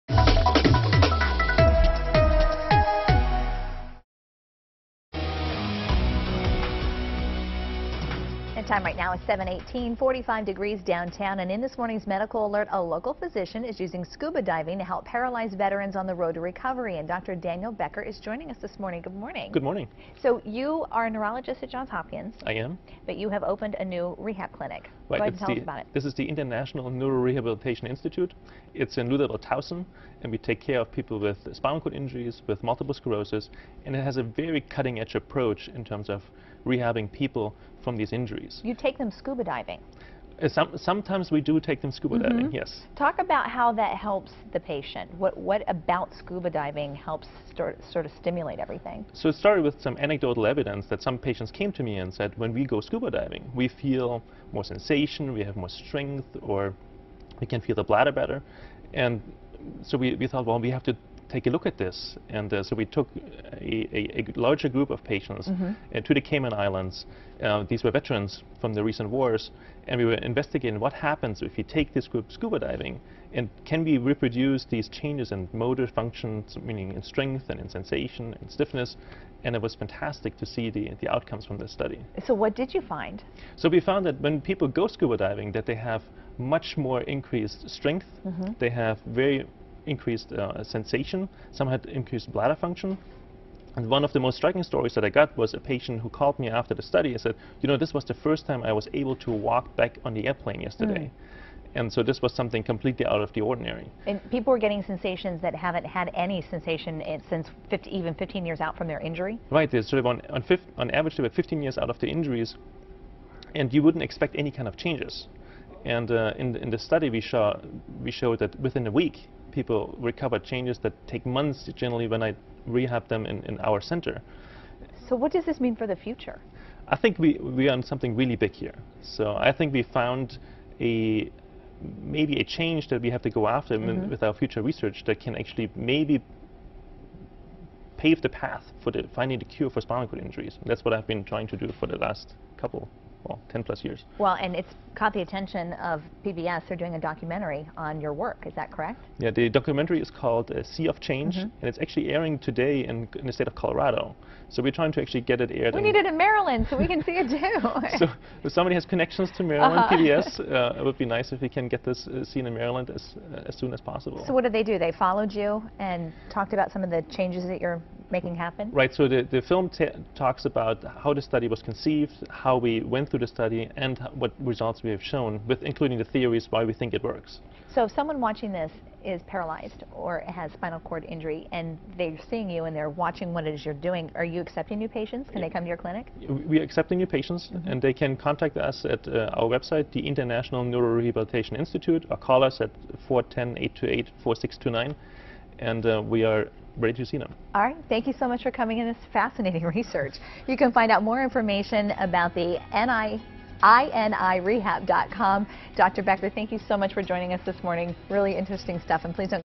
WBALTV Interview with Dr. Daniel Becker on 11/10/2013
Dr. Becker is being interviewed about the International Neurorehabilitation Institute and his experience in taking patients with spinal cord injury SCUBA diving.